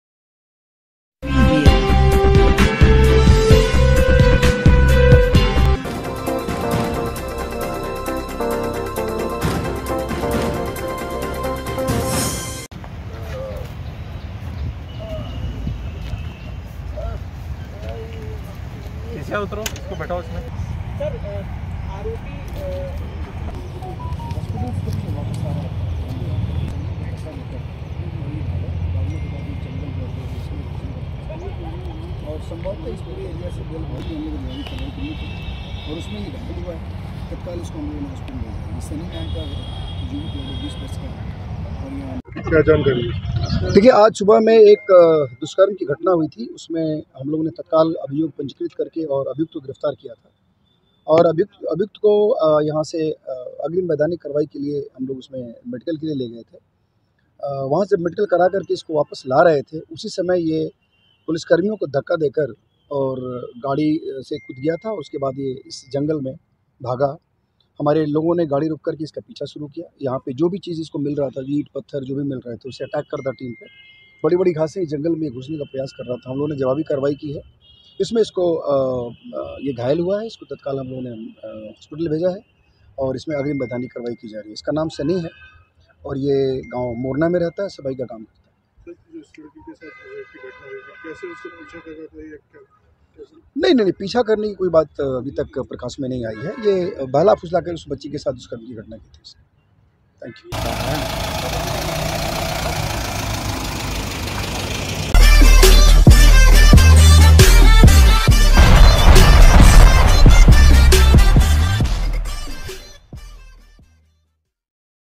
नोएड़ा रेप के आरोपी का 24 घंटे के अंदर किया नोएडा पुलिस ने एनकाउंटर 13 साल की मासूम बच्ची से रेप करके
नोएड़ा-रेप के आरोपी का 24 घंटे के अंदर किया नोएडा पुलिस ने एनकाउंटर 13 साल की मासूम बच्ची से रेप करके फरार हुआ था आरोपी

नाबालिग से रेप करने वालो को पुलिस ने एनकाउंटर में किया घायल,

13 वर्षीय नाबालिग के साथ आरोपी ने दिया था रेप की घटना को अंजाम,

थाना सेक्टर 24 पुलिस ने एनकाउंटर में किया आरोपी को घायल,